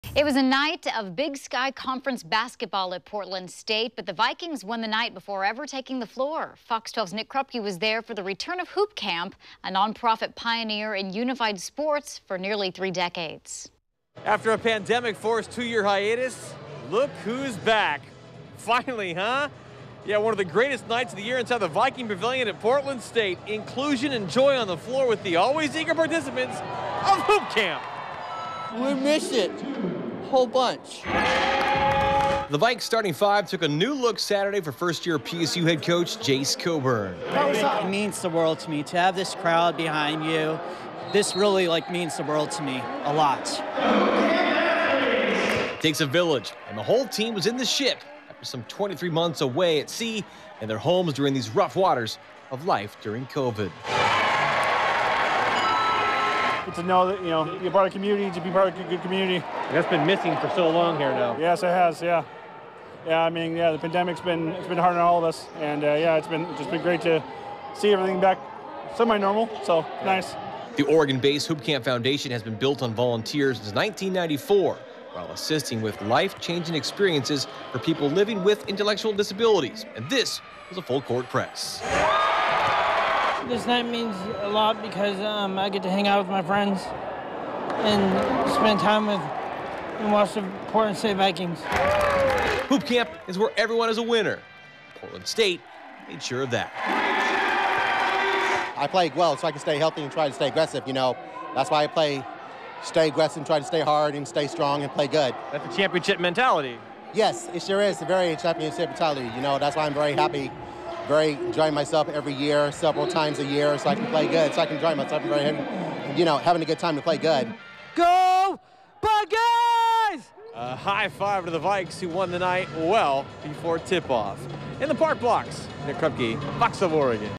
Hoop Camp returns with PSU basketball team
After a pandemic forced two-year hiatus, one of the greatest nights of the year returned inside the Viking Pavilion at Portland State.